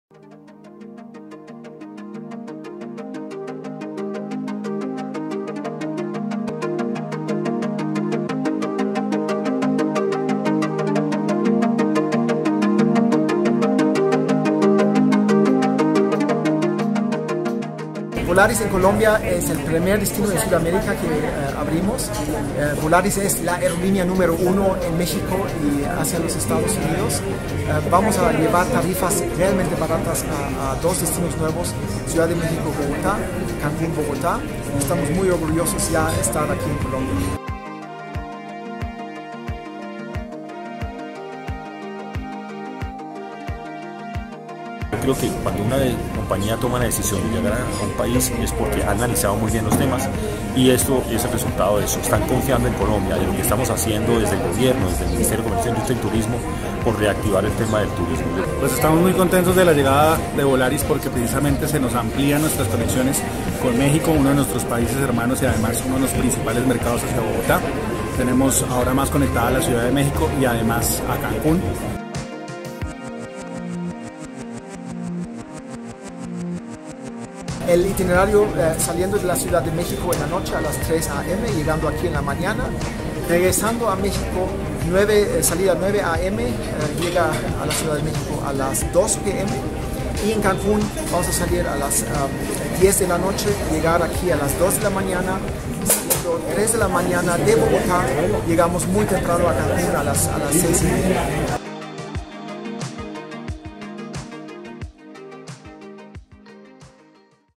Volaris numero uno en México aterriza en Colombia
La aerolínea ultra bajo costo que opera México, Estados Unidos y Centro América, aterrizó por primera vez en Sudamérica. Colombia recibio el primer vuelo de las nuevas rutas que conectaran al país con la ciudad de México y Cancún.